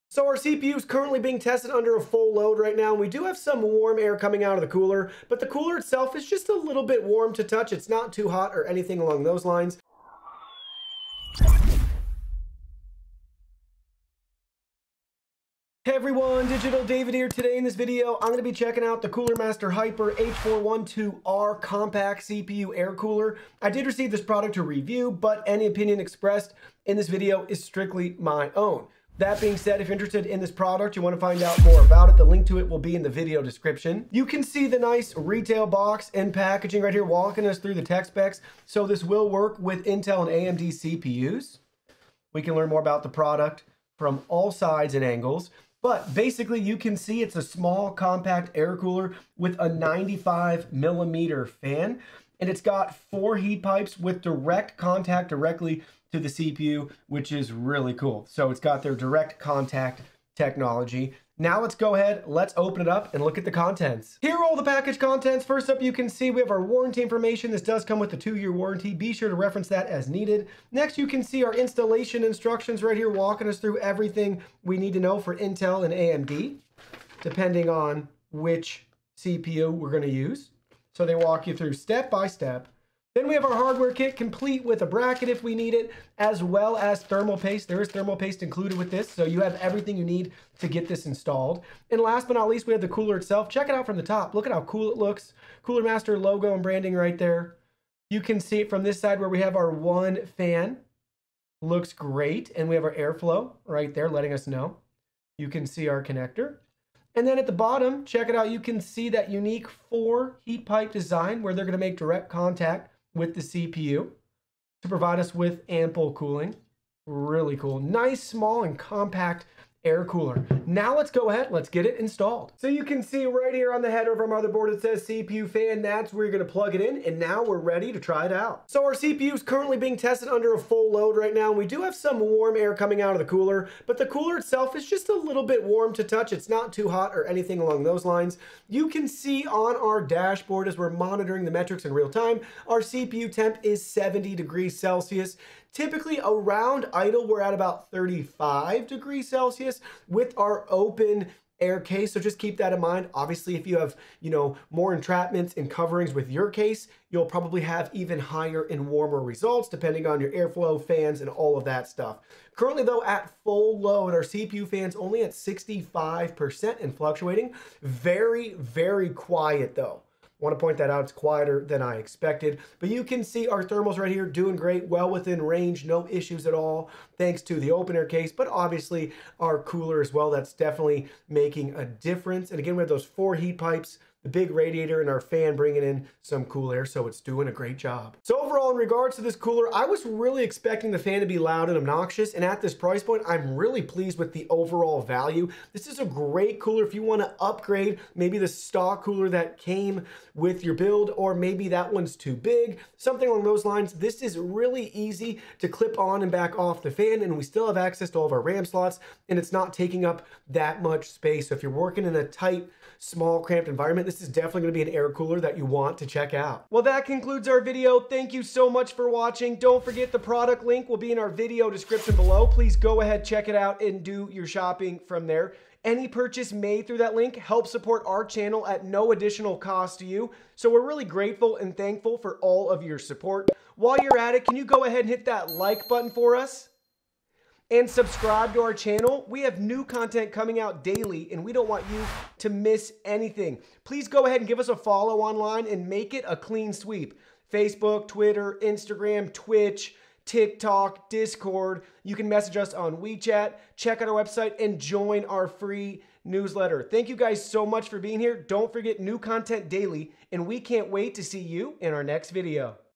Cooler Master Hyper H412R Compact CPU Air Cooler Review
Cooler Master Hyper H412R
Amazon INTL

Cooler Master Hyper H412R Compact CPU Air Cooler, 95mm PWM Fan, 4 Copper Direct Contact Heat Pipe for AMD Ryzen/Intel LGA1200/1151

00:00 - Intro
01:05 - Contents
02:02 - Installation
02:08 - Test
03:08 - Final Thoughts